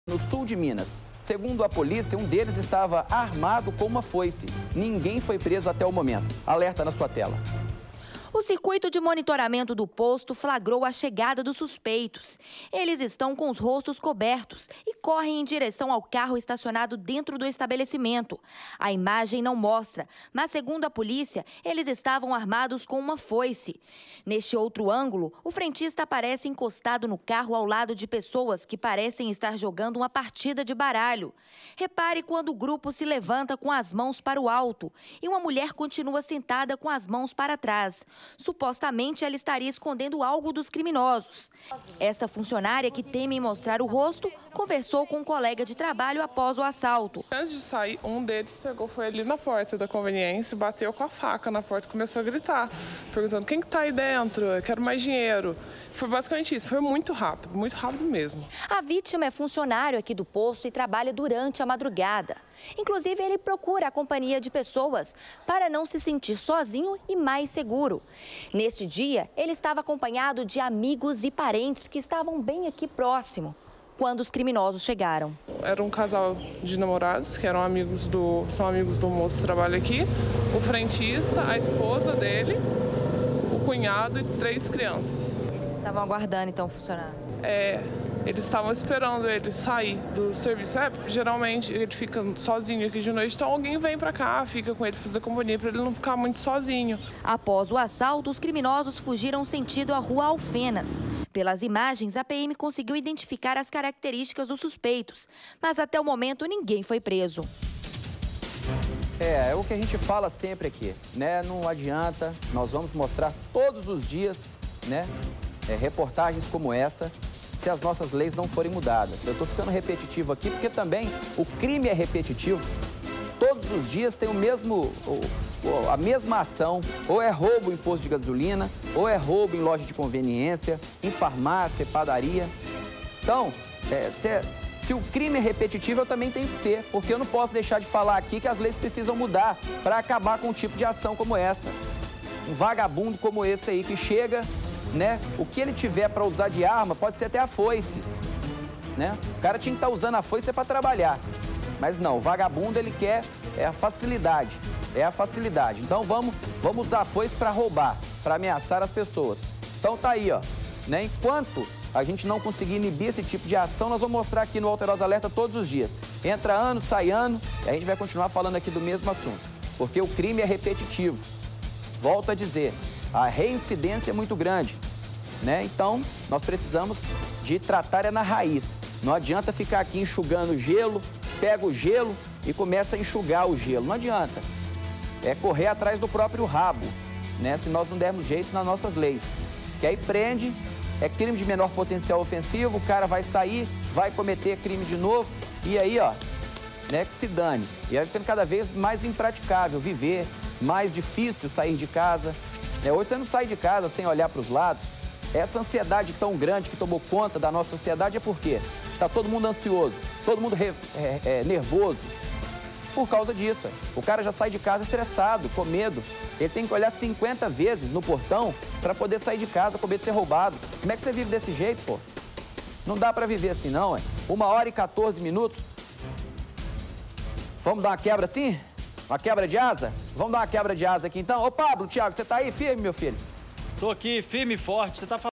Assaltante usa foice para roubar posto
O roubo aconteceu em Machado no sul de Minas.